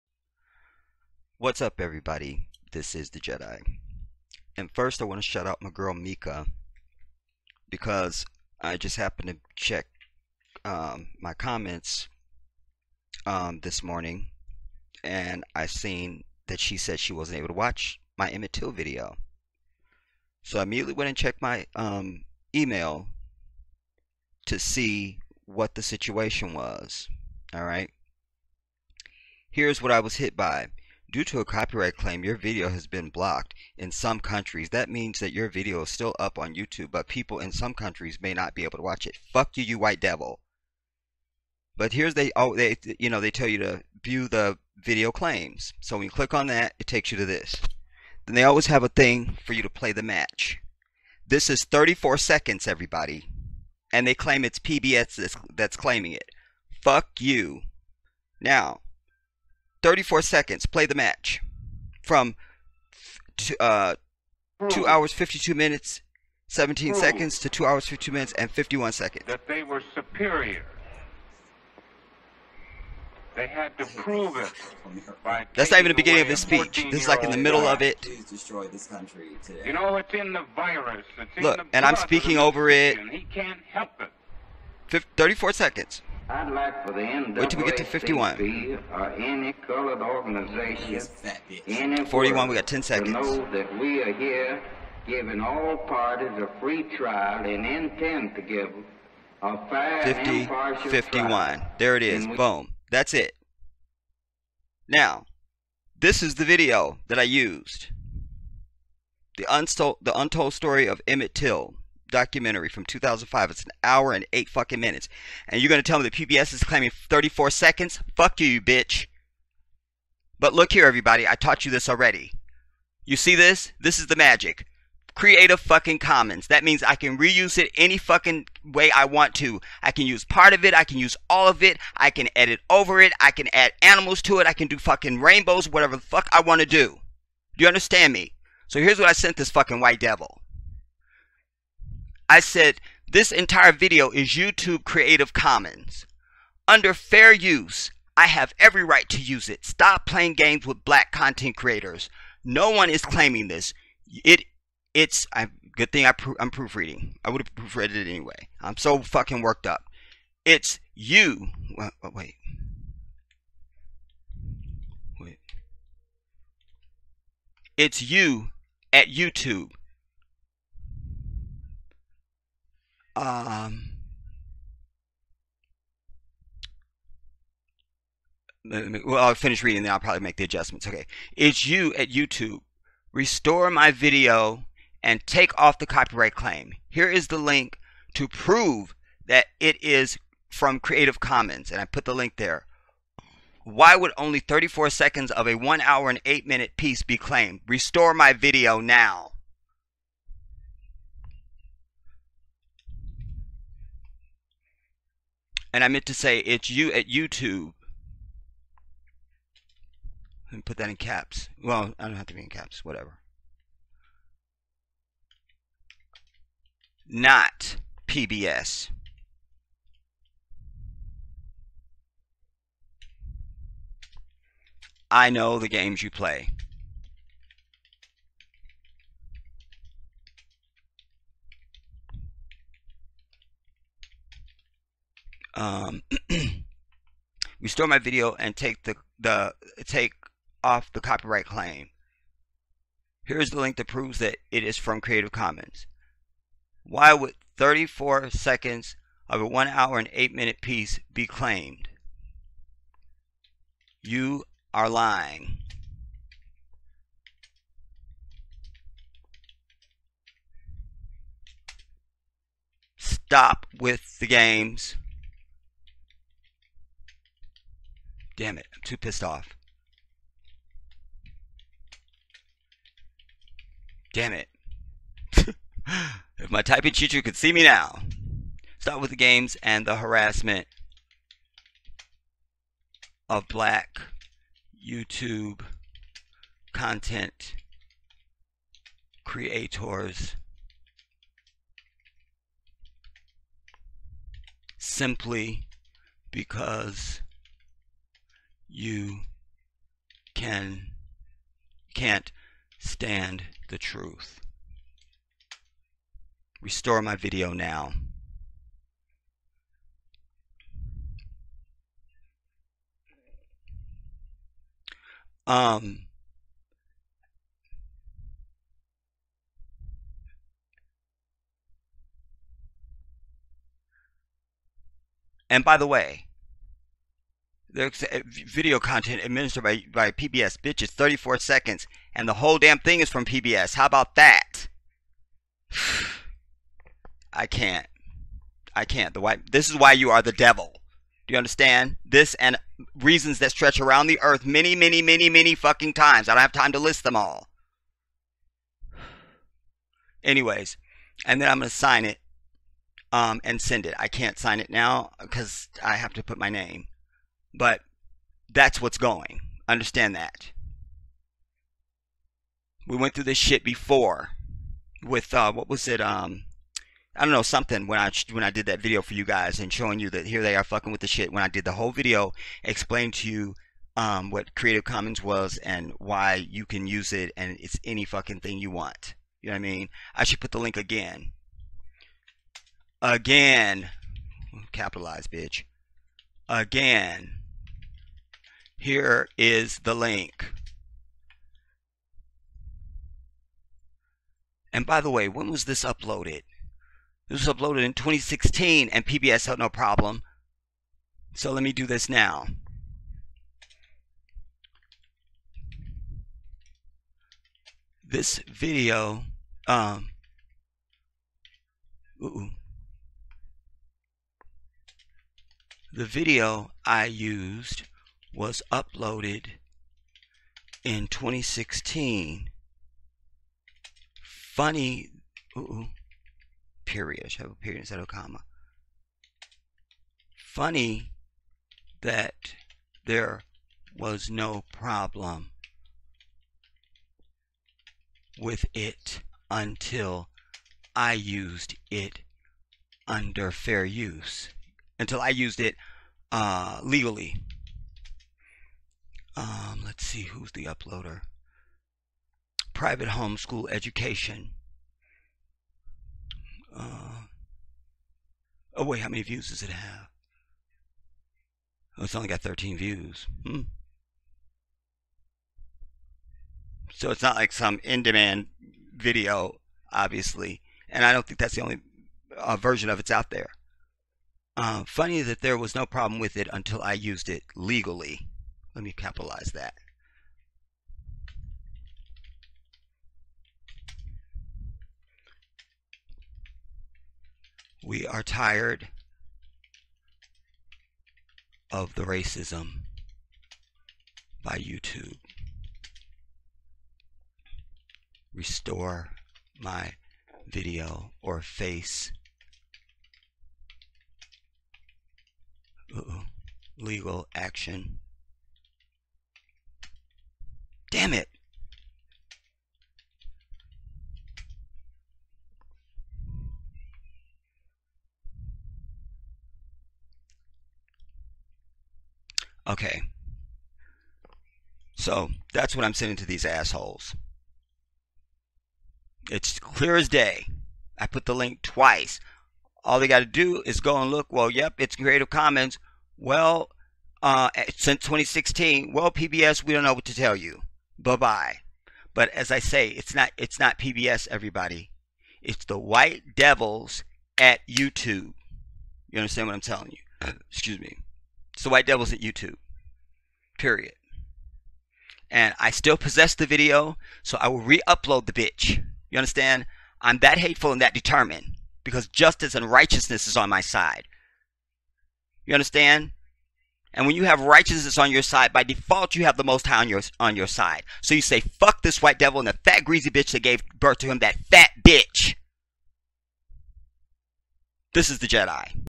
MORE ILLEGAL HARASSMENT FROM YOUTUBE SHARE THIS VIDEO NOW!!
Once again, In this society, the black man can do EVERYTHING right and the white man will go against his own rules to harass you. Listen to the details in this short video. Thanks everybody.